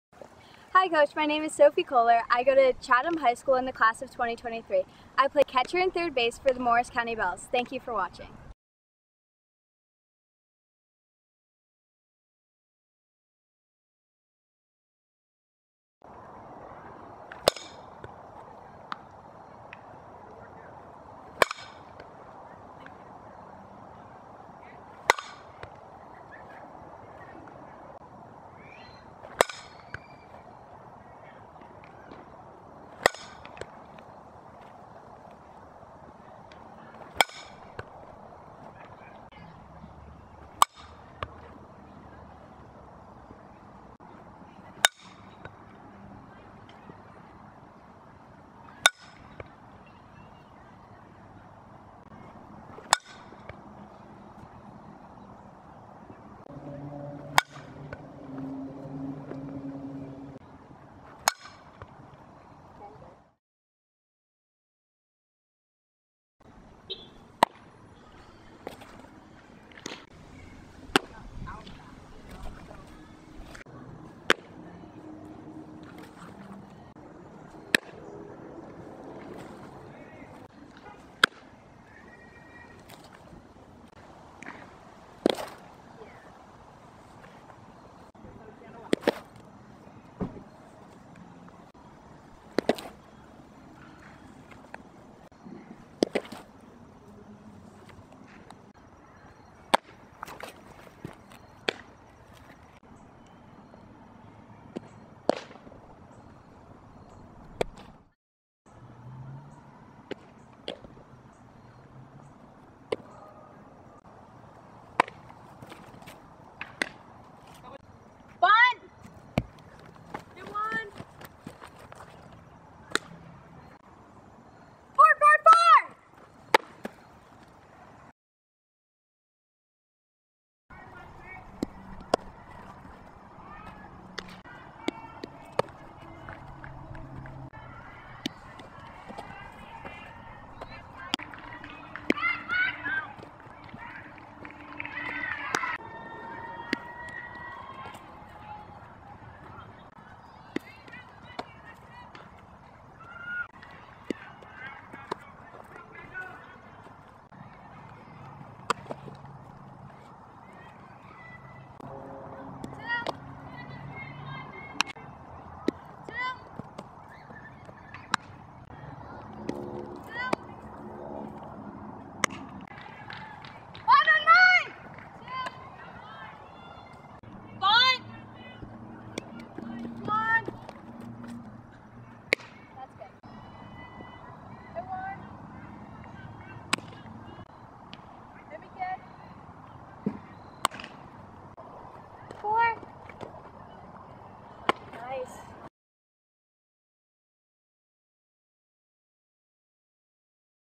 2023-C/3B Sophie Koehler Softball Skills Video
#24 Sophie Koehler
Class of 2023
Chatham High School (Chatham, NJ)
Morris County Belles 16U Showcase
C/3B
GPA: 3.9